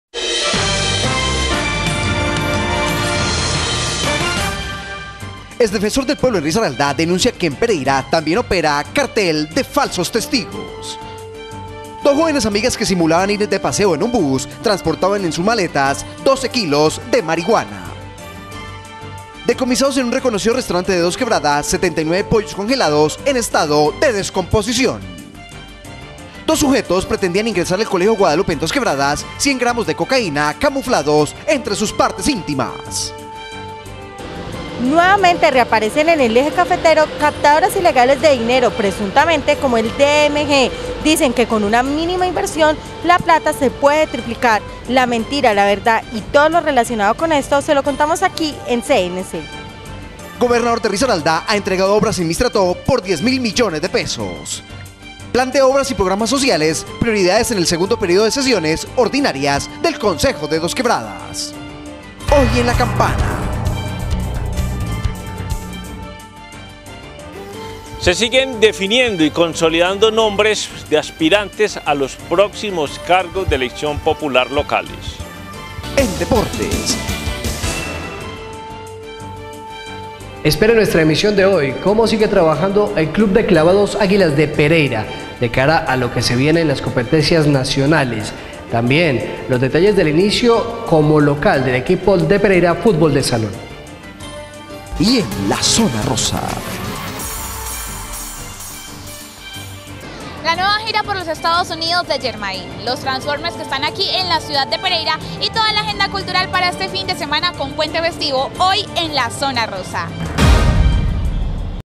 TITULARES CNC NOTICIAS VIERNES 01 DE JUNIO.
En Pereira opera cartel de falsos testigos.
*Afán por resultados judiciales.
*En el peaje de cerritos.
*El pollo farsante.
*También afectaban al Sena.
*Volvieron las pirámides a pereira.
*Educación y seguridad.
*Reconocieron labor del alcalde.
*Talento artístico de exportación.